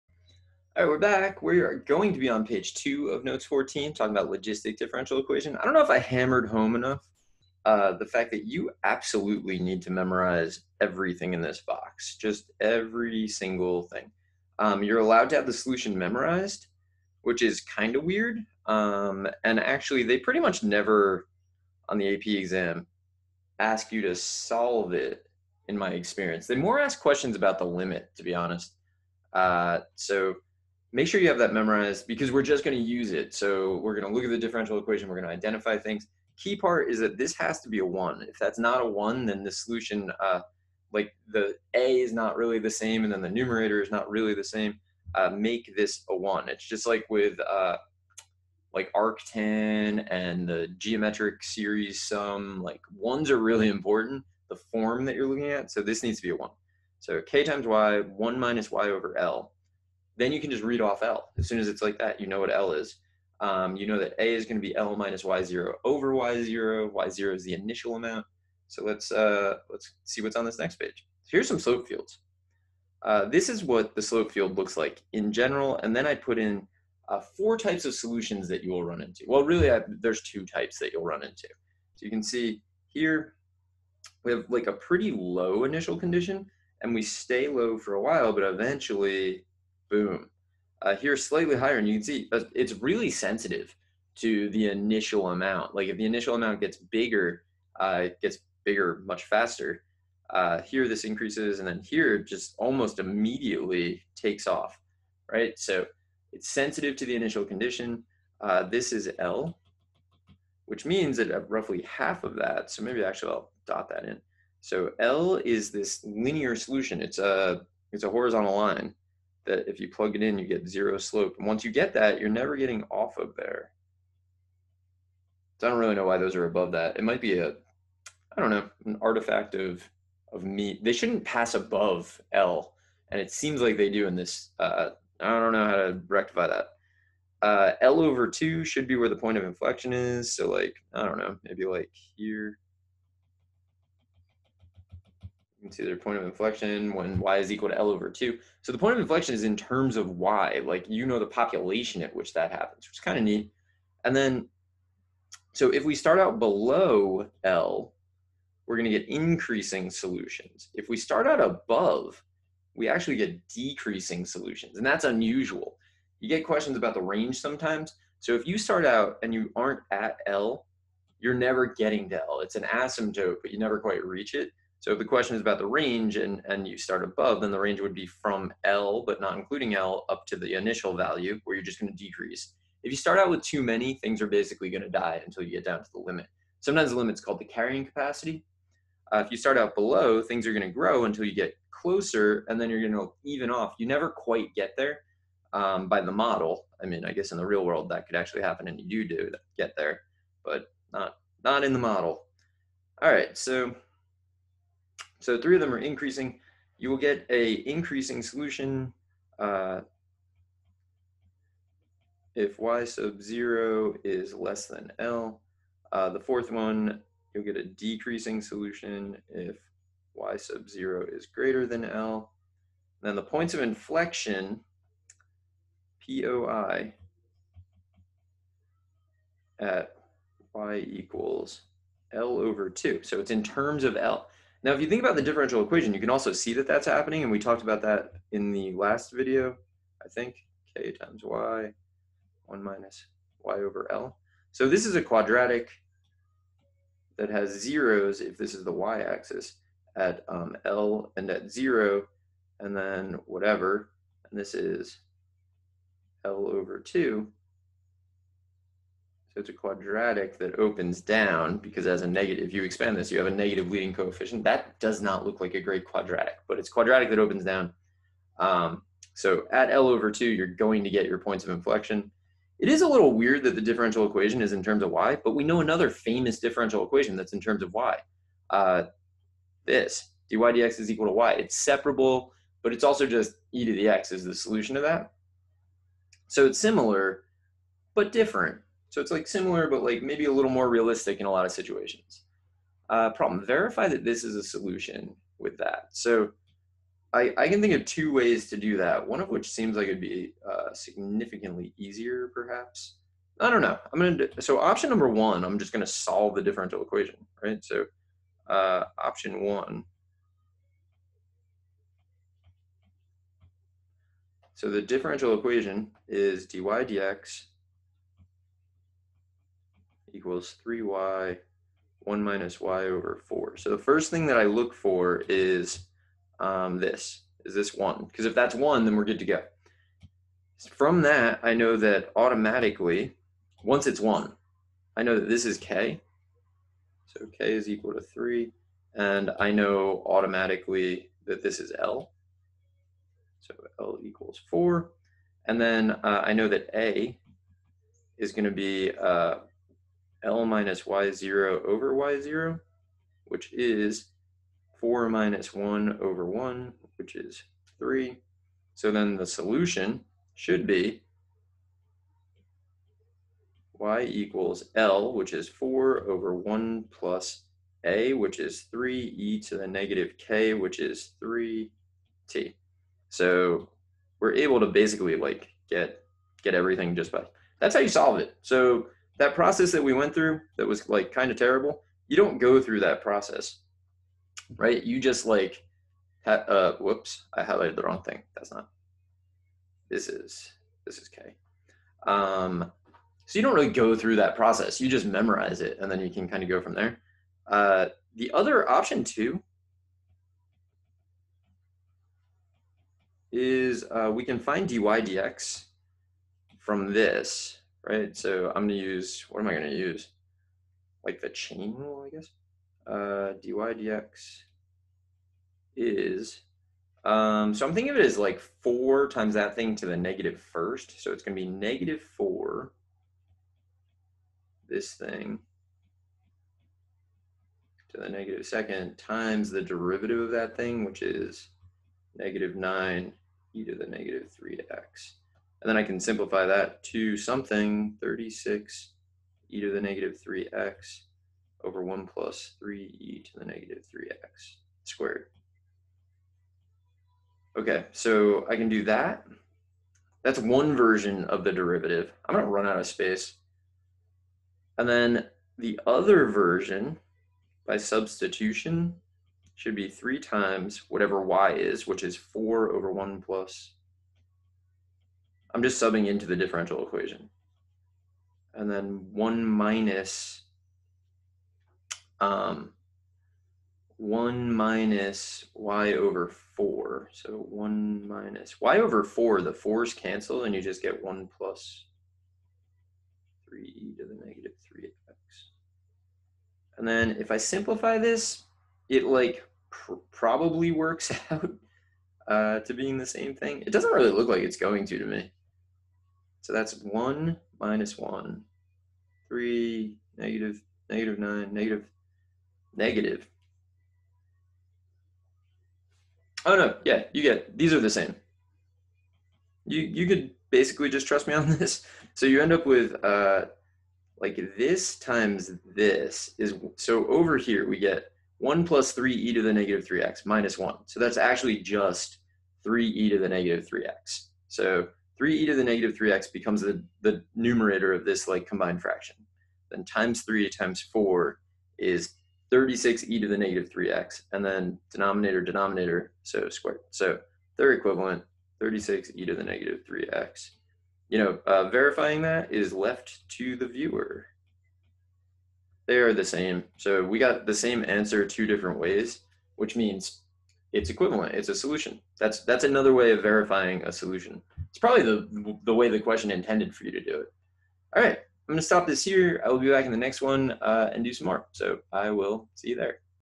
Slope Field for Logistic Differential Equation pg 2
Calc C Notes 14, pg 2

Topics: slope field for logistic differential equation; increasing solutions; decreasing solutions; range of solution; verifying a solution by solving; verifying a solution by substituting